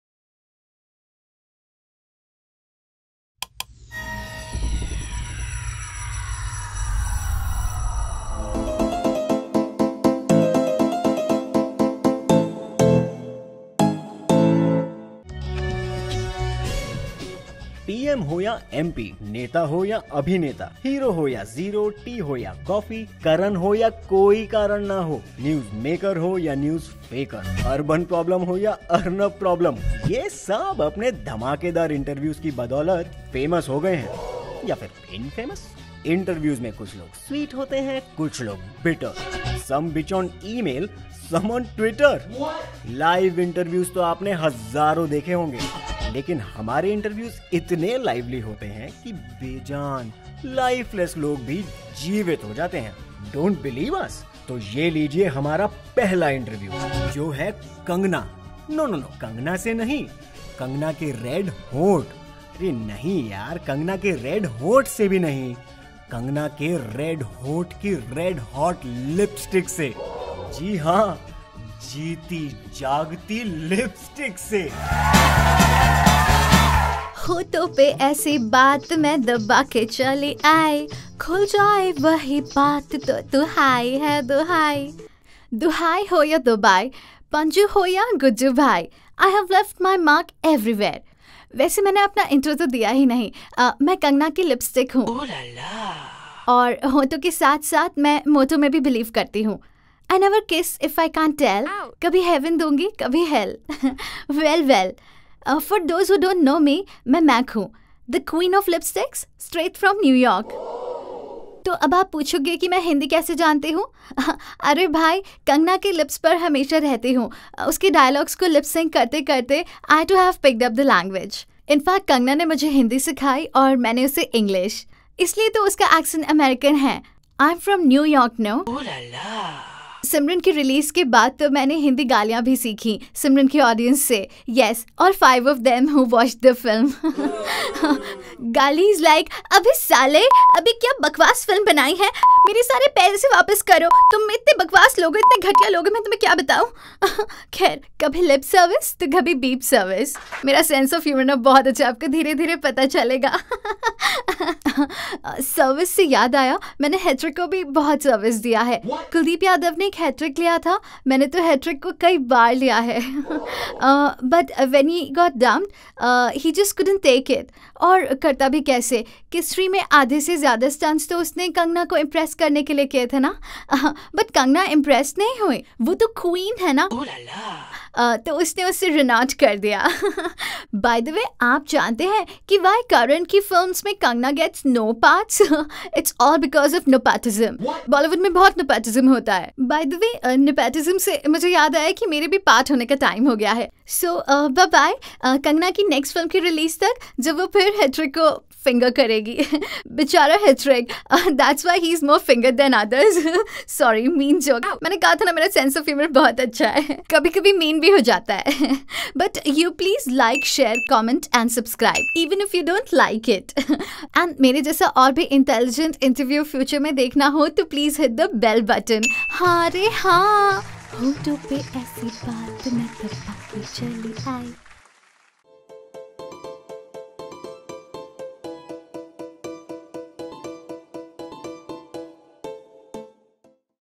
INTERVIEW WITH KANGANA'S LIPSTICK I SPOOF I ANIMATION
How often have you heard the phrase  "If only the walls could talk"? Well, In our world they CAN  talk. As can a whole bunch of inanimate objects.We're launching a talk show starring such objects which have heard things that no one else has. And now they are sharing it just with you. Exclusively on our just launched Web series 'Kuchh Bhi Bakta Hai…' Our first celeb guest is red hot Kangana's even redder and hotter LIPSTICK! Please do listen to her… even if she talks Kuchh bhi…

Produced by : pretty short films.
Directed by : Pushkar Sinha
Written by : Manish Jhaveri
Animation, Graphics & Edit : Realpix studio
Voice Over Artists : Vijayta Saran & Anurag Jha